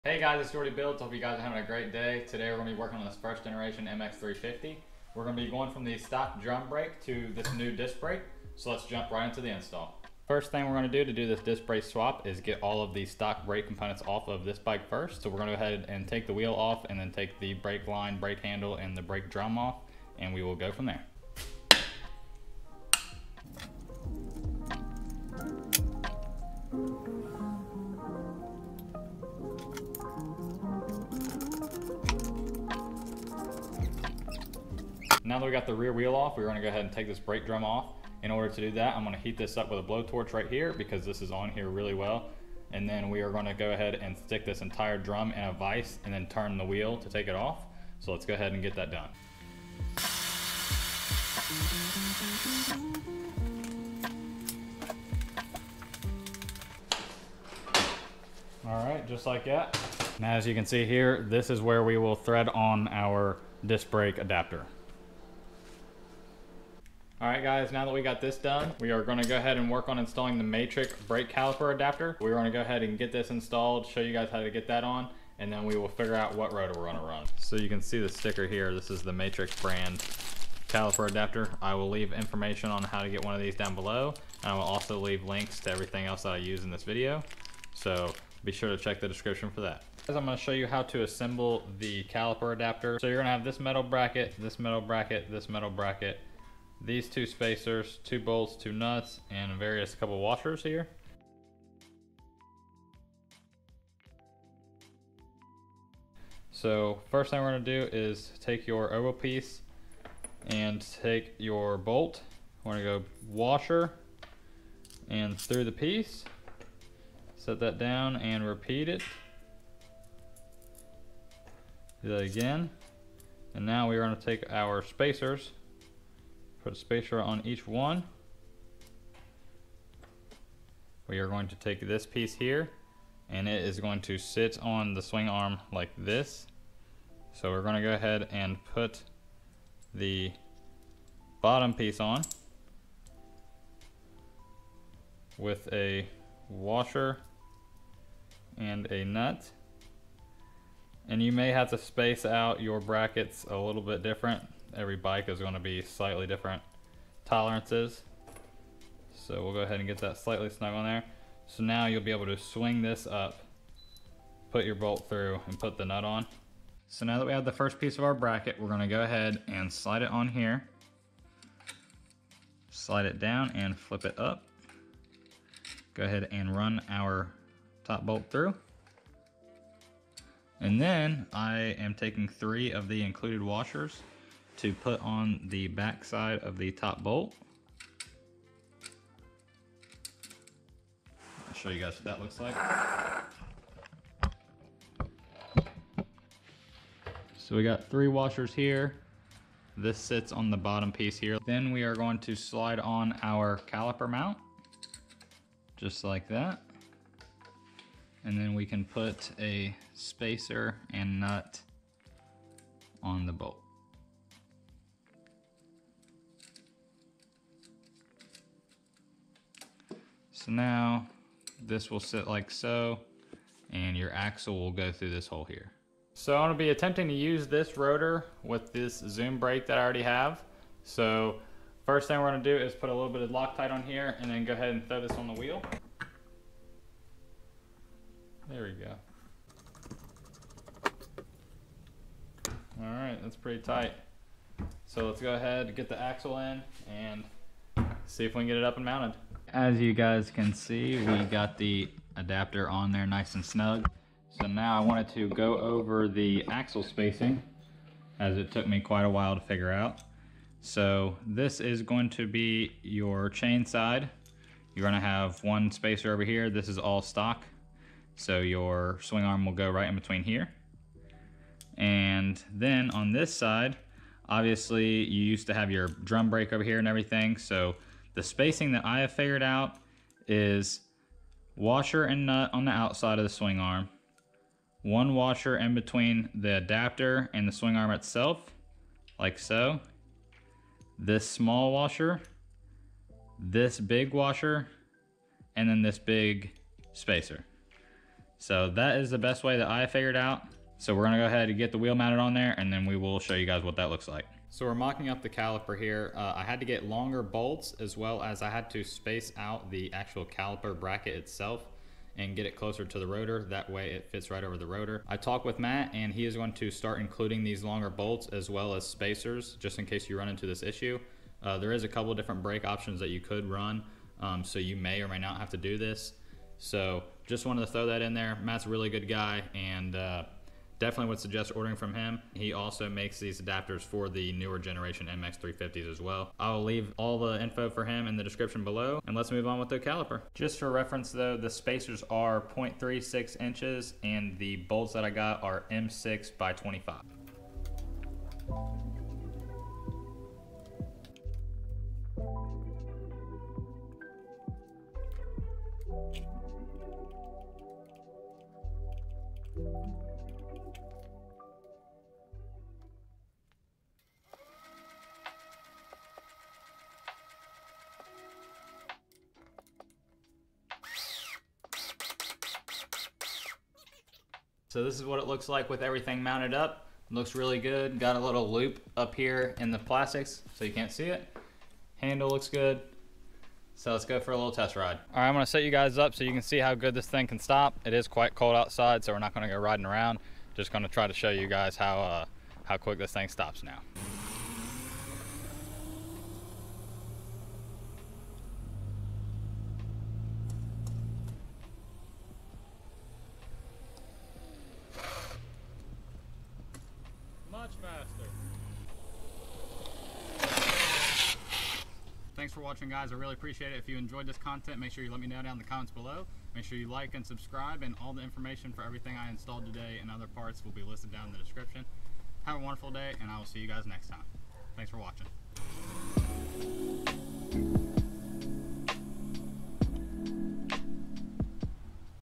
How to Install Disk Brakes on Razor MX350 First Gen
Hey guys today we go over the install for the Matric Disk Brake adapter kit for the first generation mx350.
He also makes them for the pocket mod, newer mx350, etc!

Brake and rotor used in video